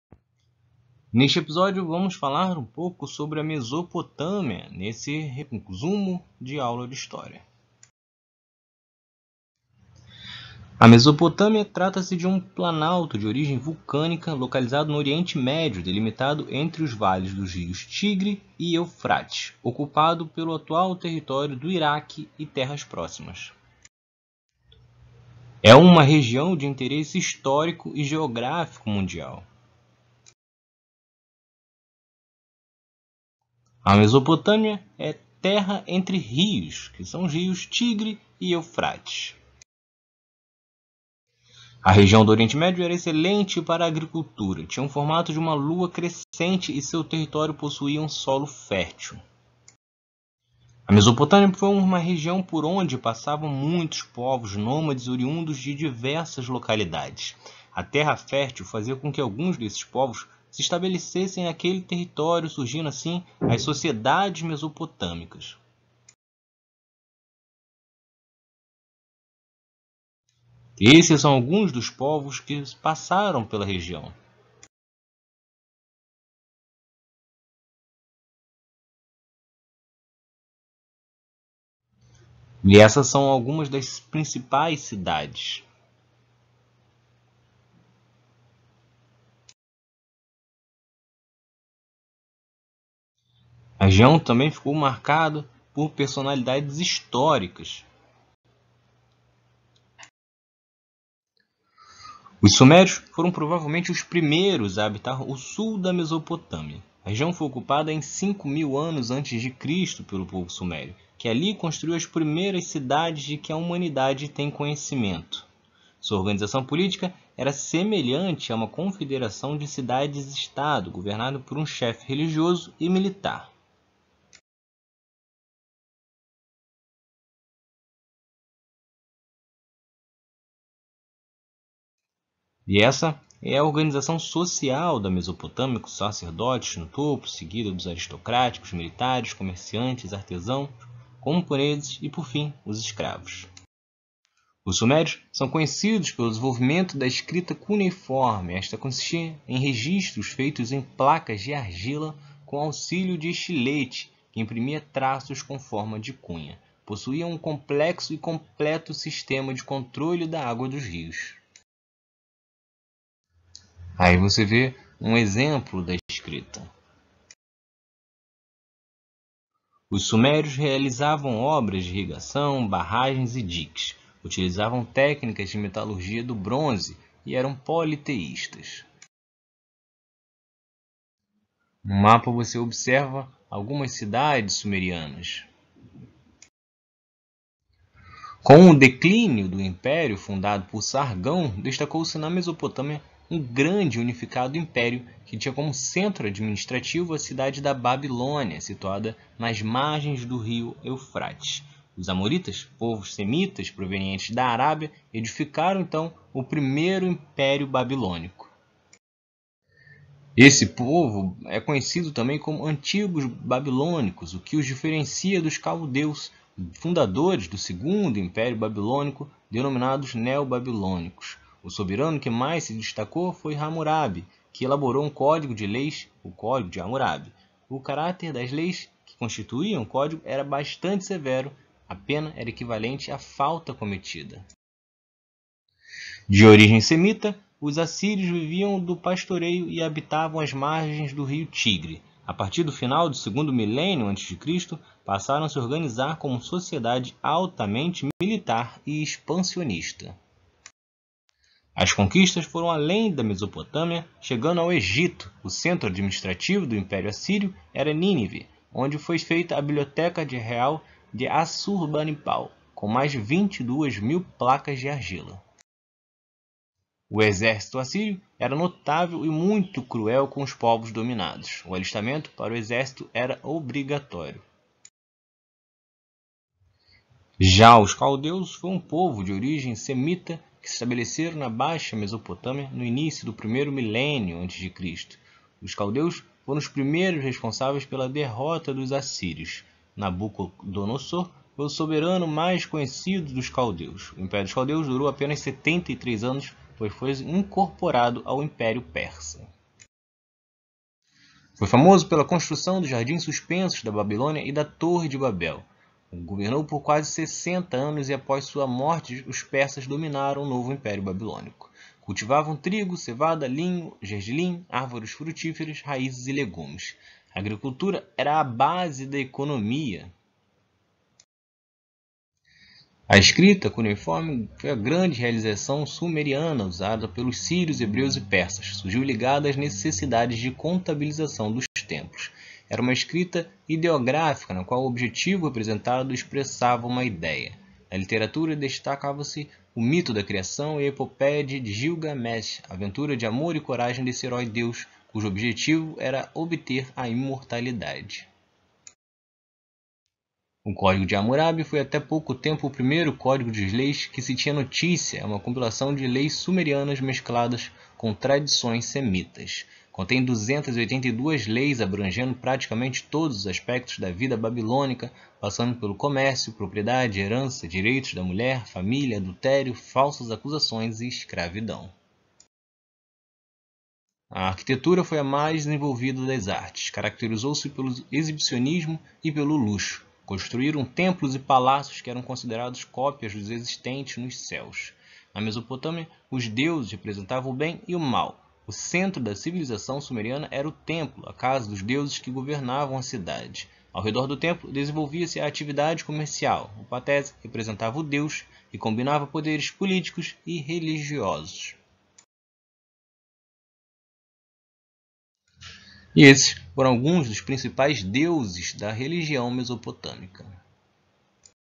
Mesopotâmia - Resumo de aula
Este episódio é um conteúdo de apoio, para reforço escolar de estudantes. O conteúdo sobre a região da Mesopotâmia é normalmente aplicado no 6º ano do Ensino Fundamental.